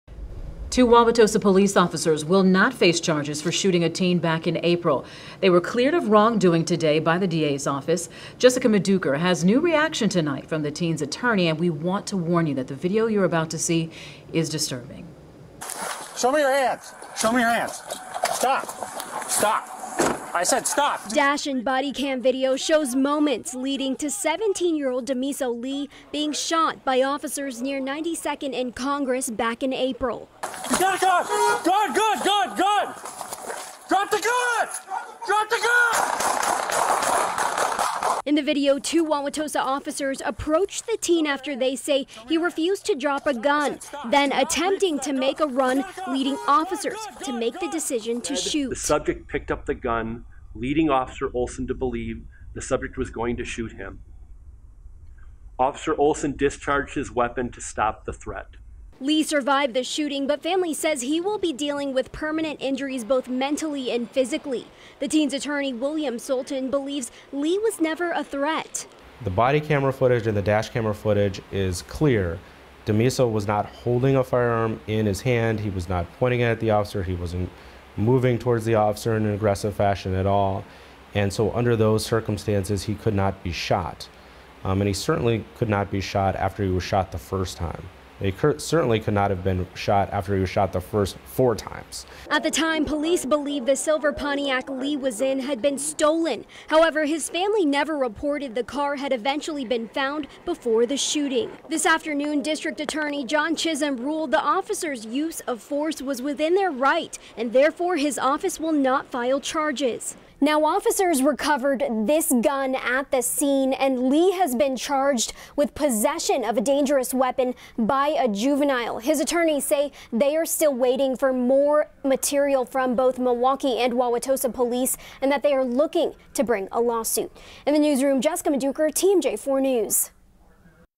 Body cam video released in Wauwatosa police shooting of teen during stolen car investigation
Body camera video was released Monday, providing a first look at an officer-involved shooting that happened in Wauwatosa in April.